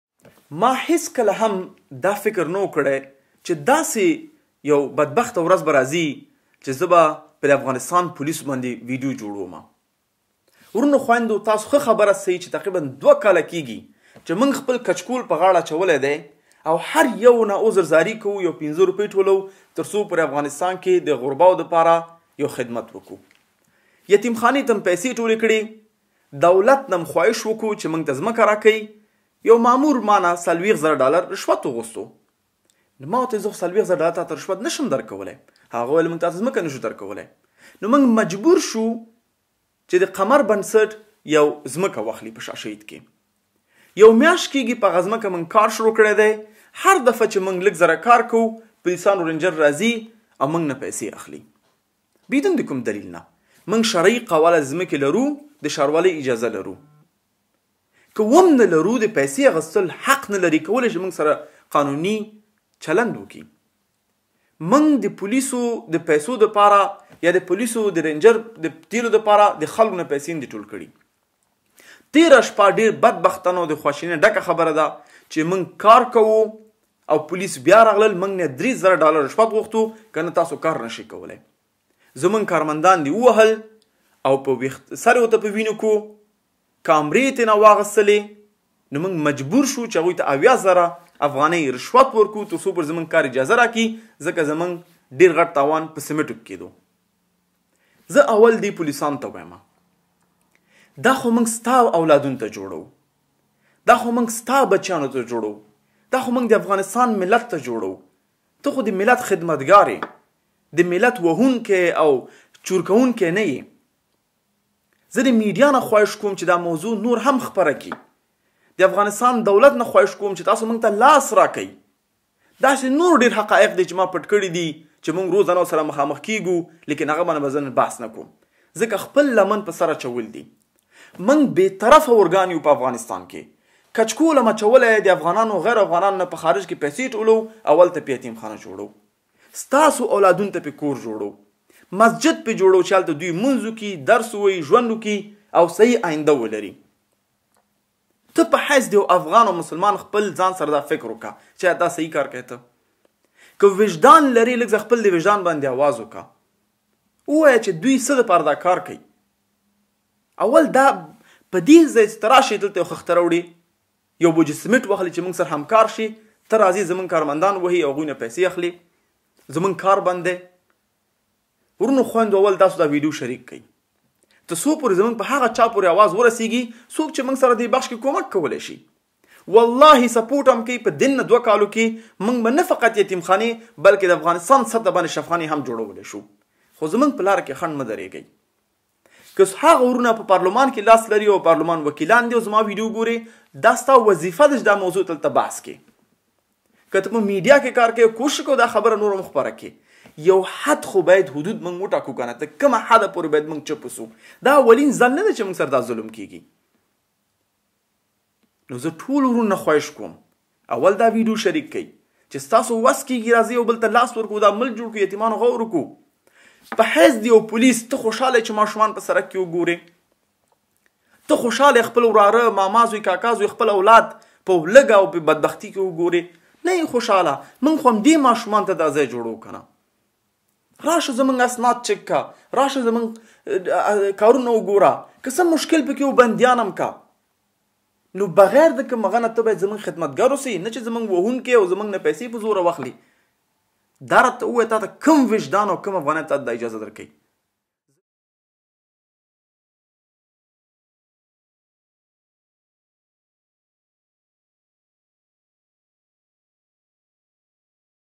پوليسو لخوا يې کارونه ځنډول کيږي.
لکه څرنګه چې تاسې ته بهتره معلومه ده چې د قمر بنسټ لخوا په کابل ولايت کې يتيم خانه جوړيږي او کارونه يې په جريان کې دي خو بدبختانه د پوليسو لخوا يې کارونه ځنډول کيږي.
مونږ له هغوی ټولو څخه هيله کوو چې پدې نيک کار کې له مونږ سره مرسته وکړي تر څو د لارو کوڅو يتيمانو ته د زده کړې او اوسېدو ځای برابر کړو او سبا ورځ همدوی د وطن د ابادۍ لپاره کار وکړي.
دا چې د يتيم خانې لپاره پيسې او هر څه د عامو خلکو کومک دی او د کومک کوونکو پيسې بايد د يتيم خانې په ابادۍ مصرف شي تر څو د هغوی ورکړې پيسې صدقه جاريه شي، نه دا چې د يو چا جيب ته لاړې شي.